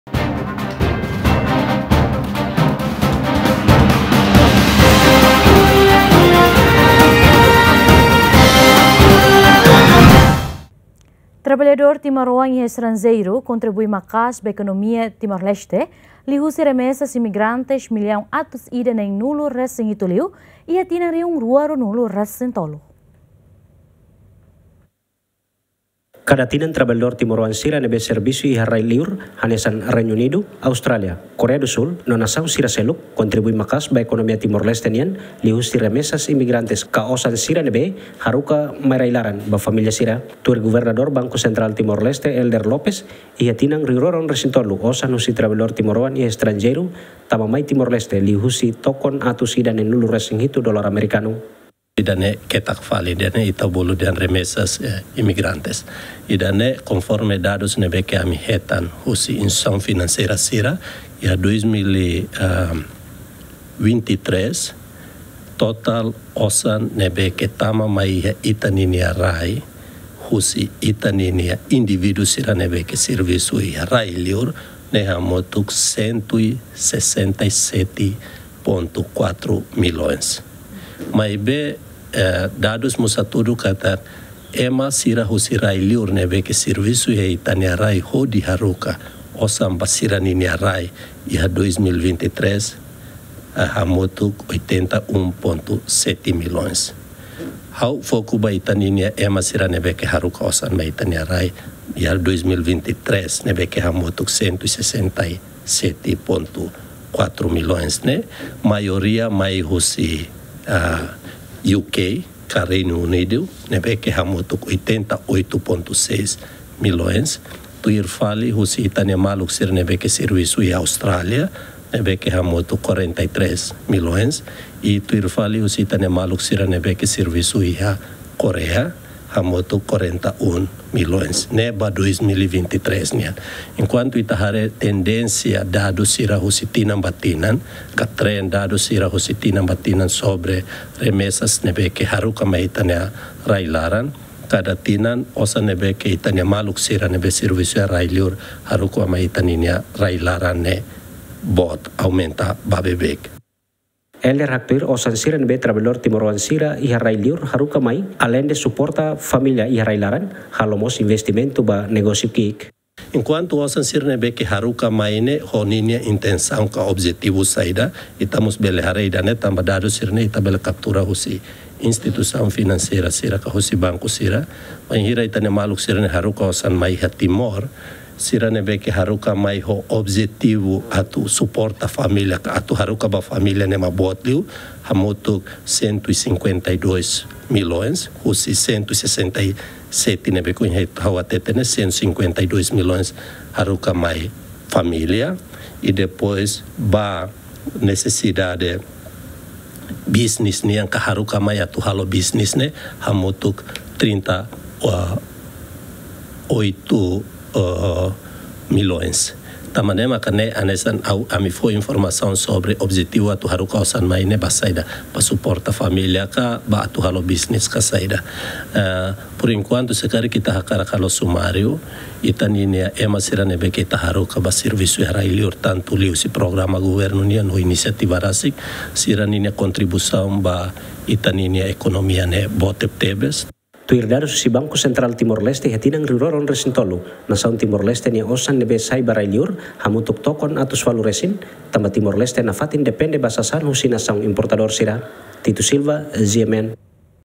Traballador Timoroan iha estranjeiru kontribui maka´as ba ekonomia TL liu husi remesas imigrantes
Traballador Timoroan iha estranjeiru kontribui maka´as ba ekonomia TL liu husi remesas imigrantes Millaun 167 liu iha tinan 2023